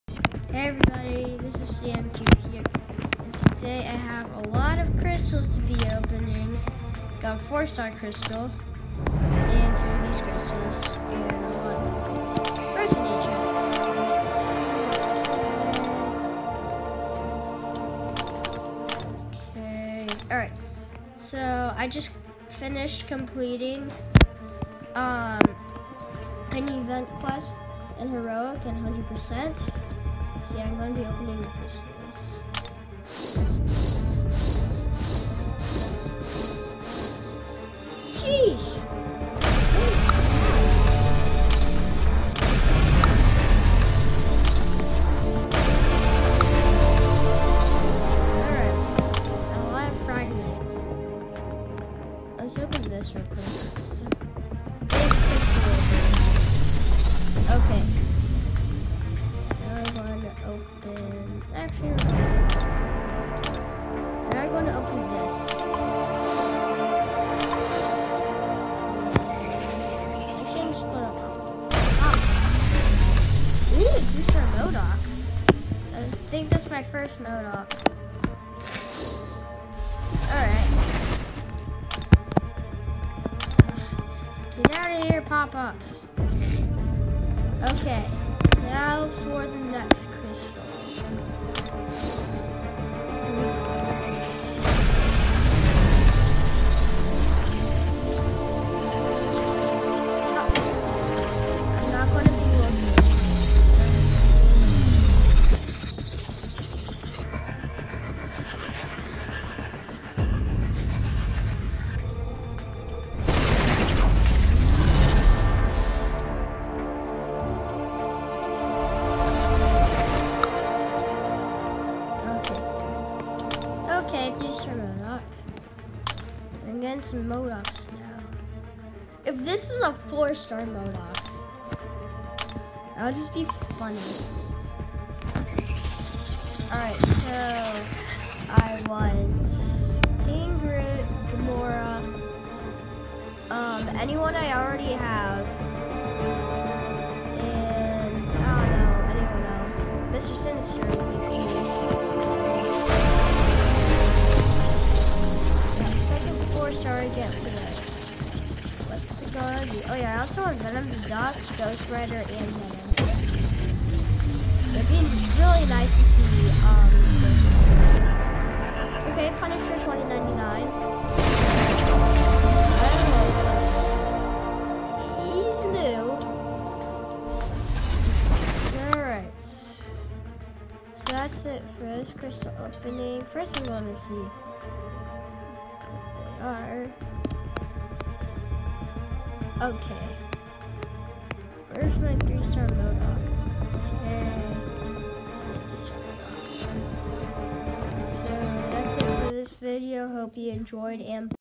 Mega crystal opening
I beat the monthly event quest in heroic and this is the results